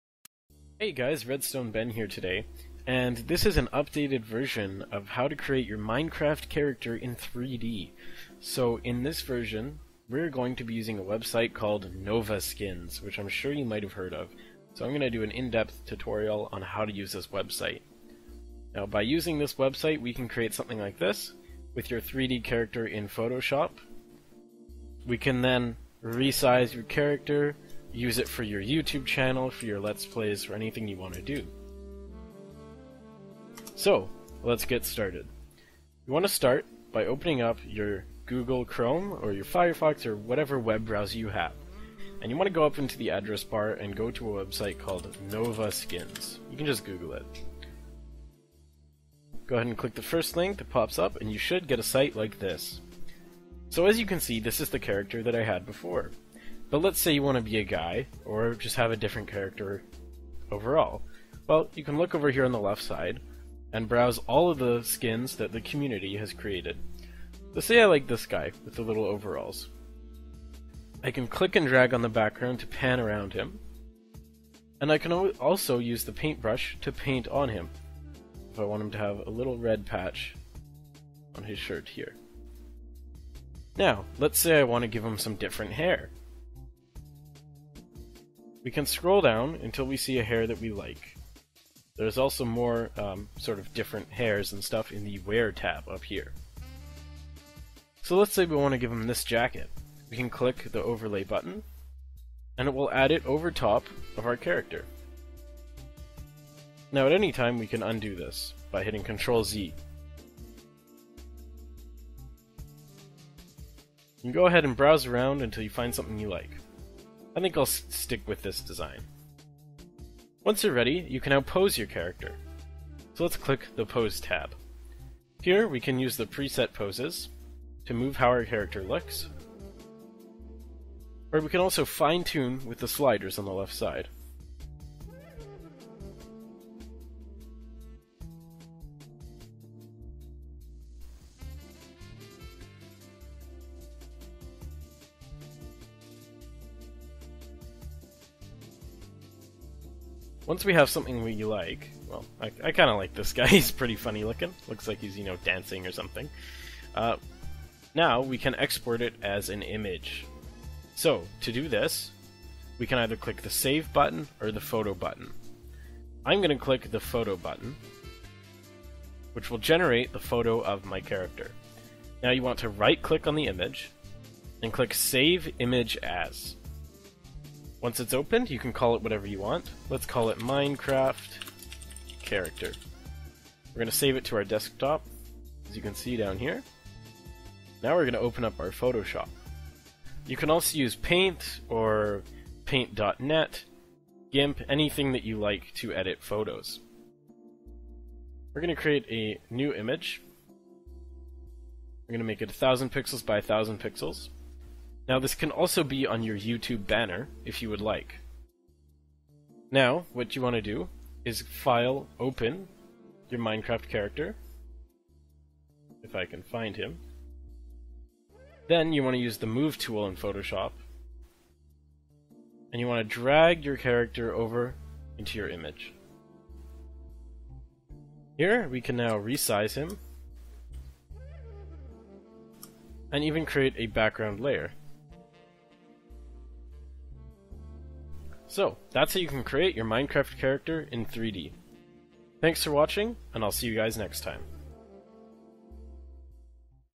How to Make Your Minecraft Character in 3D! (UPDATED)
How to render your character in 3D so you can add it to your logo or background, UPDATED VERSION!